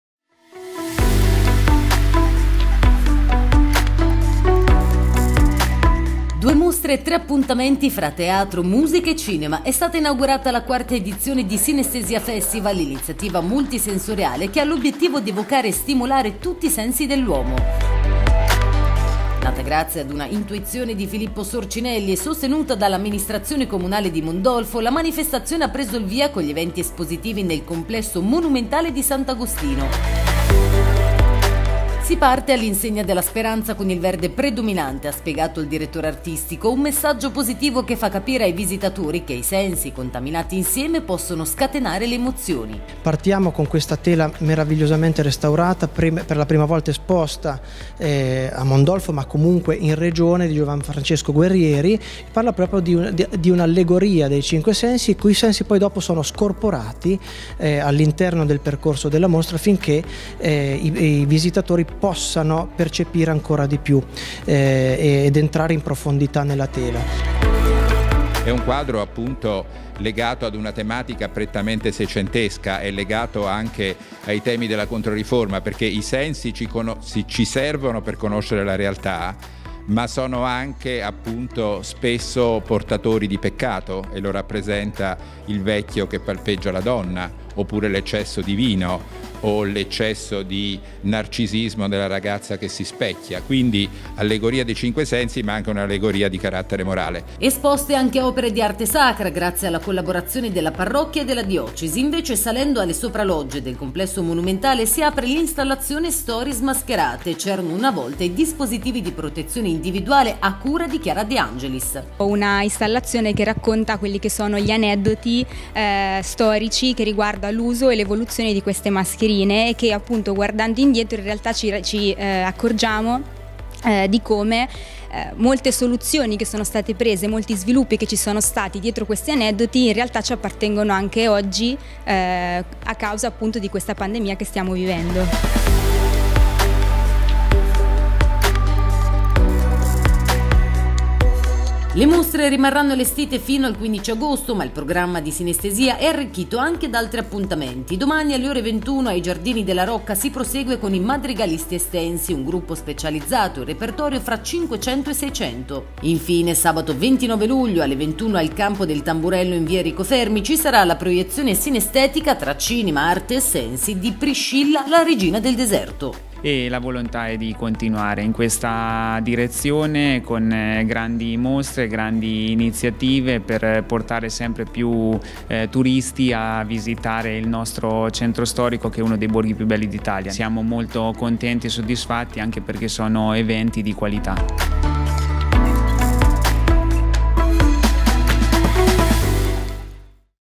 Mondolfo: al via Synesthesia Festival
A Mondolfo è partita la 4ª edizione di “Synesthesia Festival”.